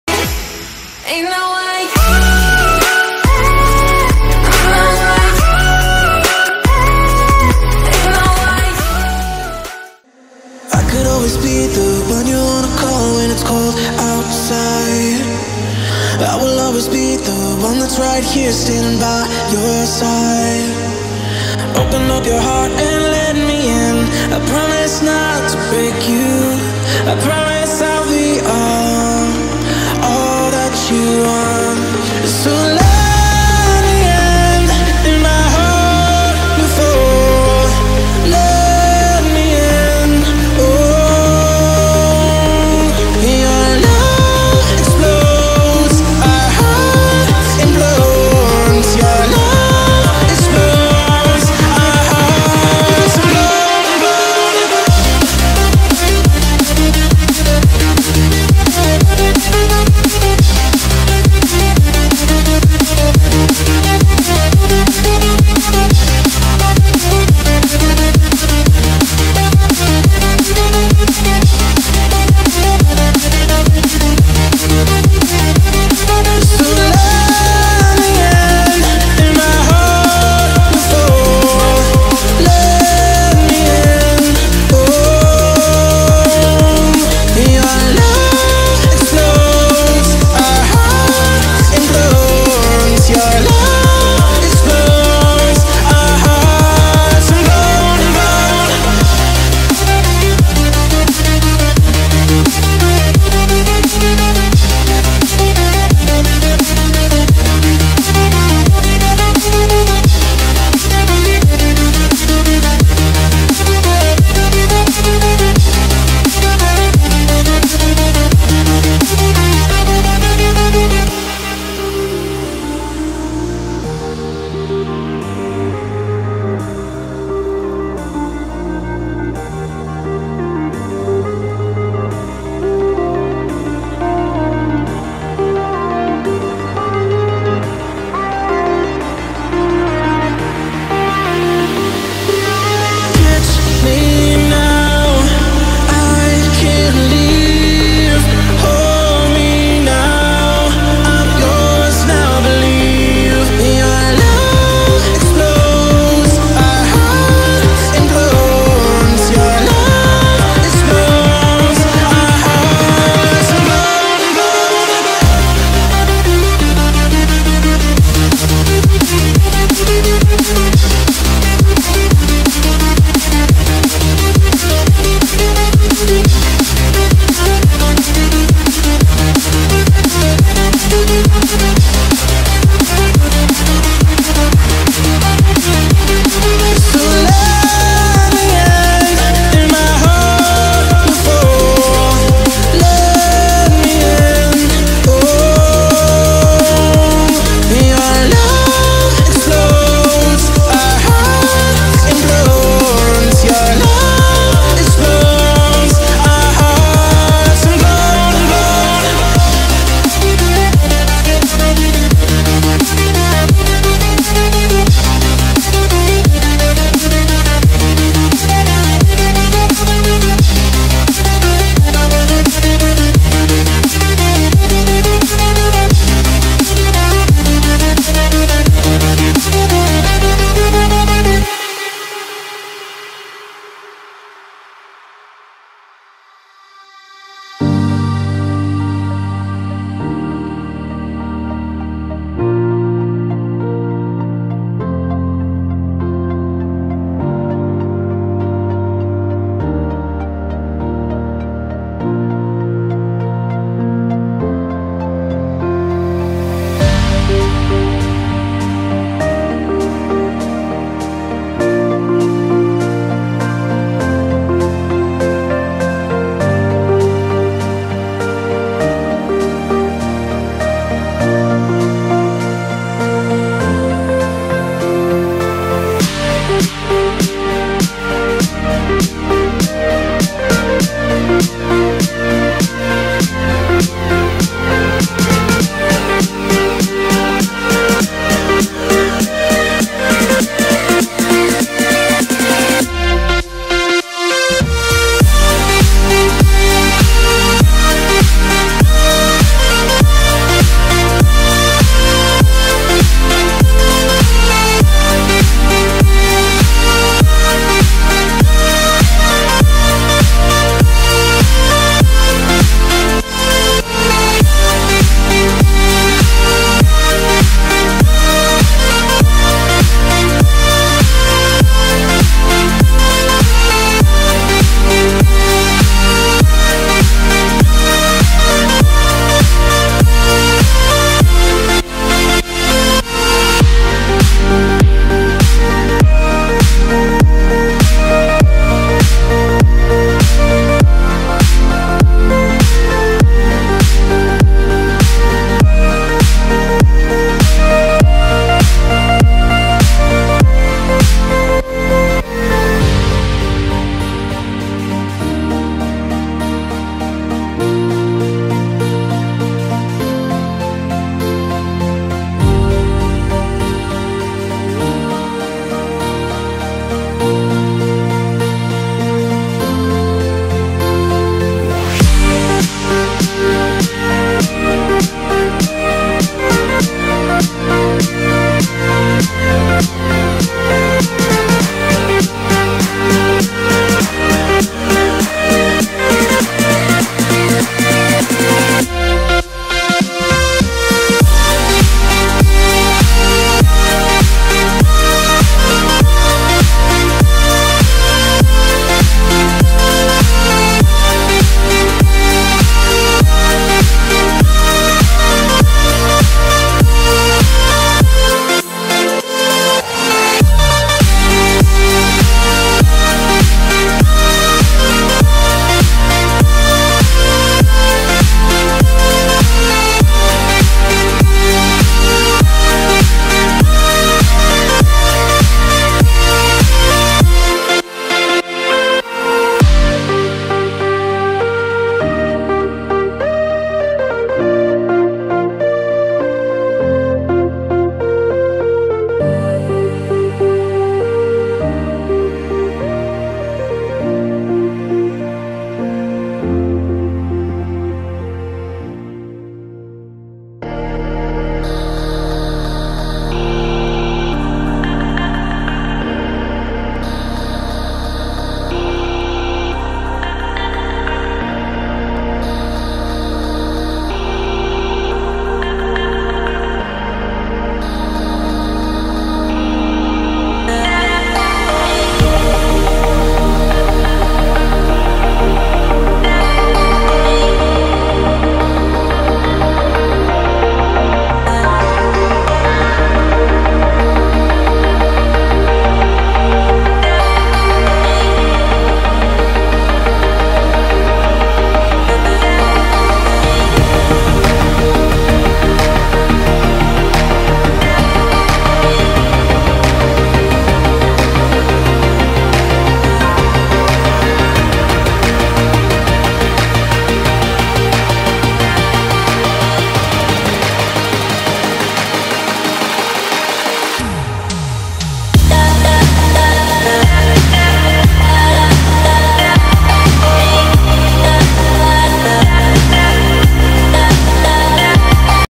VORTEX IMPERTINENCE/HARDI - SADI/PANDA/SRAM/ENI - FT VOLCASAURUS! FORCE OF NATURE = CHEAT!!!!!!!!!
Force of Nature, a 5 ap strength spell available to sadidas wrecks all the mobs when proximity poison is applied, doing increased damage per tree to a maximum of 121-127 base earth on a crit when 5 grown trees are anywhere on the map! Cheat af!

Shoutout to my boy Volca who played the panda/sadi in this fight!

Give me a follow for op set ideas utilising current endgame meta gear for pvp and pvm!